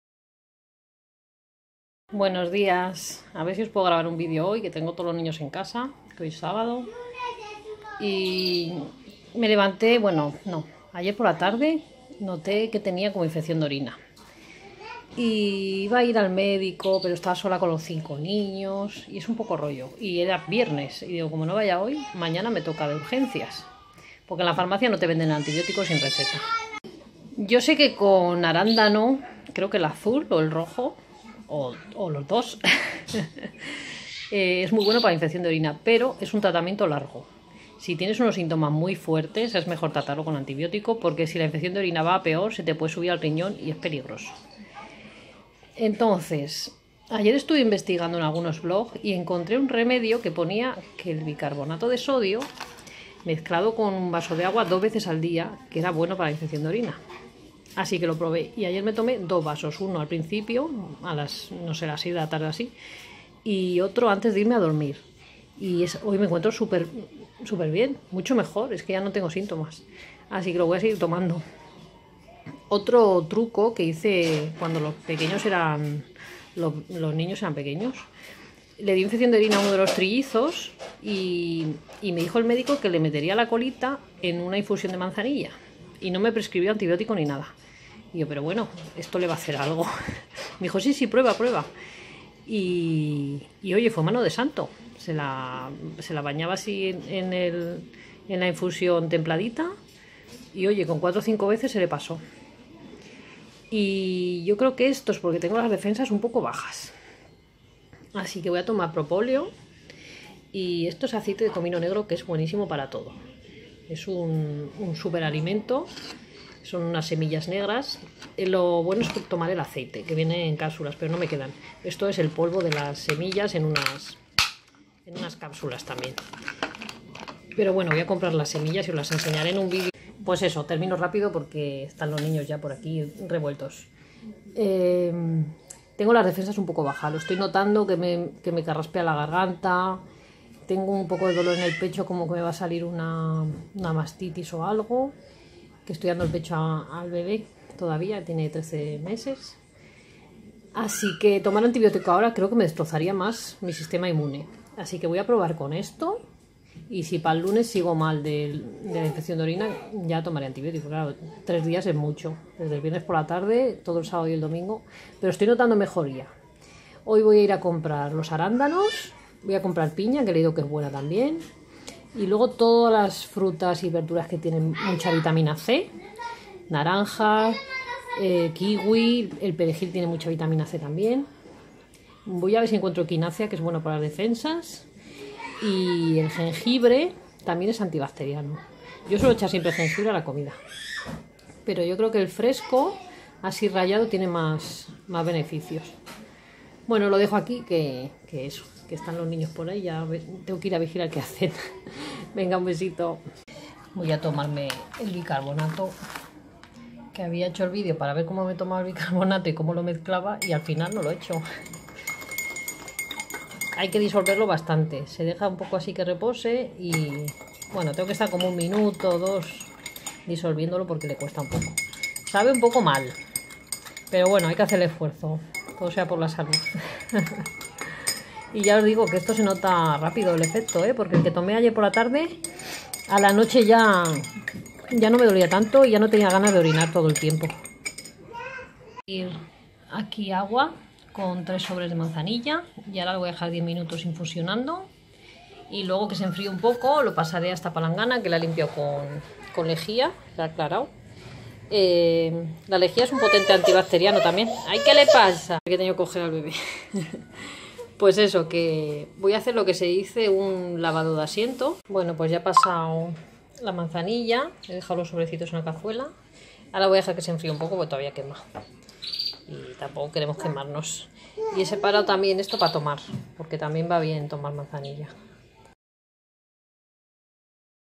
Remedios naturales para la infección de orina: bicarbonato, arandanos, manzanilla,...
Desde ayer estaba notando síntomas de infección de orina y con estos remedios estoy notando mucha mejoría.  Os dejaré en un comentario si conseguí eliminarla del todo o si he tomado antibiótico finalmente. ❤
Aceite de comino negro en cápsulas

Tintura de propoleo

Mi movil Samsung Galaxy A5: